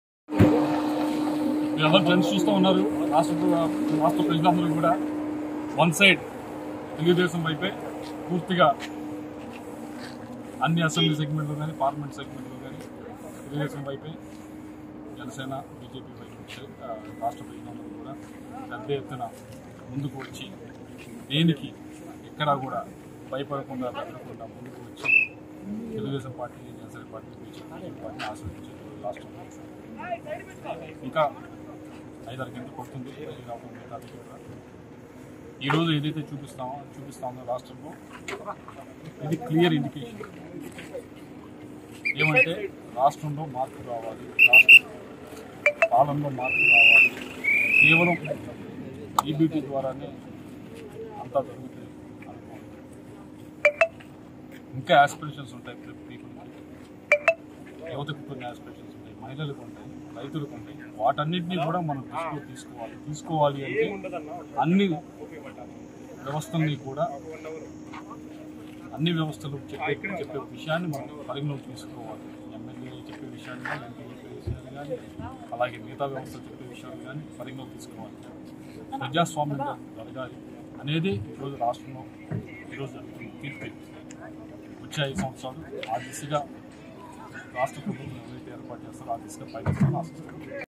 ఏపీ ప్రజలు మార్పు కోరుకున్నారు: లావు శ్రీకృష్ణదేవరాయలు
ఏపీలో వార్‌ వన్‌ సైడ్‌ అయిందని, అన్ని అసెంబ్లీ, పార్లమెంట్‌ స్థానాల్లో ఎన్డీయే కూటమి ఆధిక్యం కొనసాగుతోందని నరసరావుపేట తెదేపా (TDP) ఎంపీ అభ్యర్థి లావు శ్రీకృష్ణదేవరాయలు అన్నారు. ఎన్నికల ఫలితాలు చూస్తే ఆంధ్రప్రదేశ్‌లో ప్రజలు మార్పు కోరుకున్నారని అర్థమవుతోందని స్పష్టం చేశారు. కూటమి ప్రభుత్వంలో ఏపీ అభివృద్ధి చెందాలని ఆకాంక్షించారు.